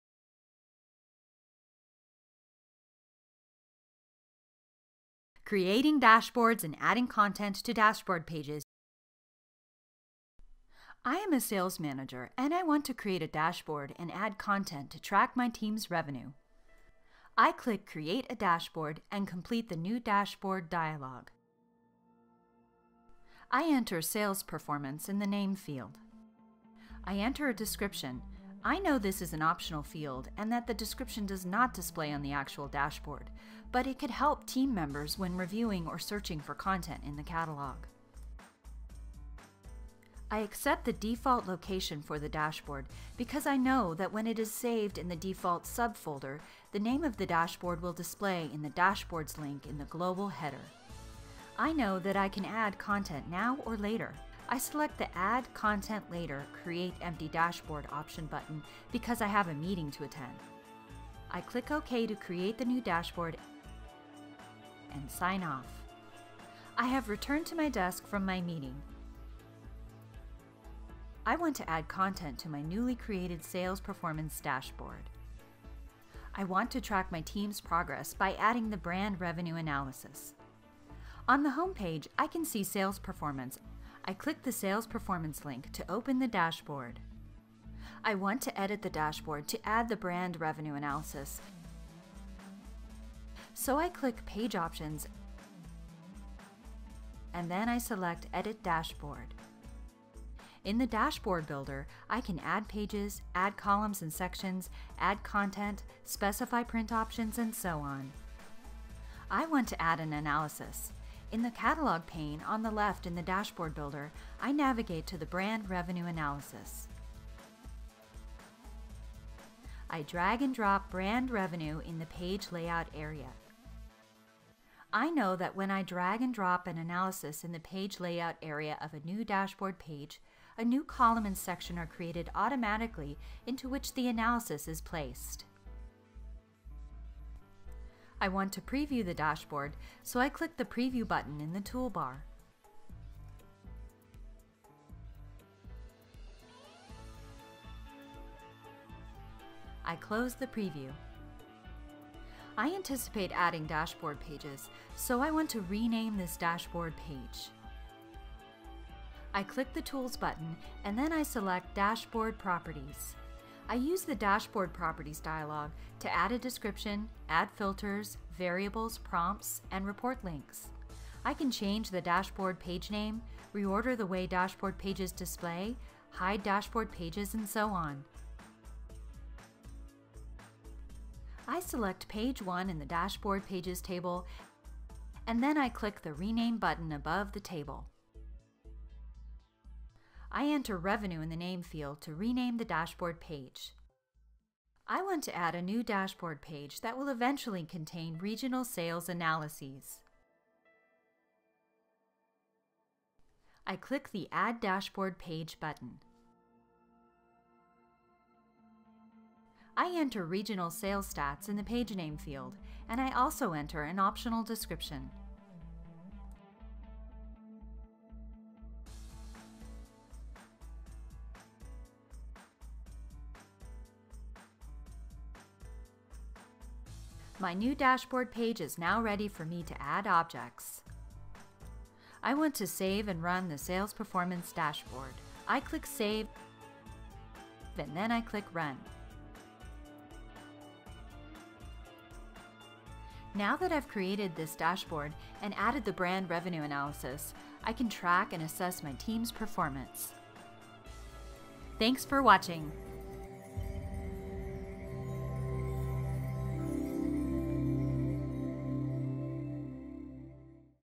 Creating Dashboards and Adding Content to Dashboard Pages Using Oracle BI EE
This video teaches you how to create a dashboard and add content to dashboard pages.

To find out more about Oracle BI EE, see the documentation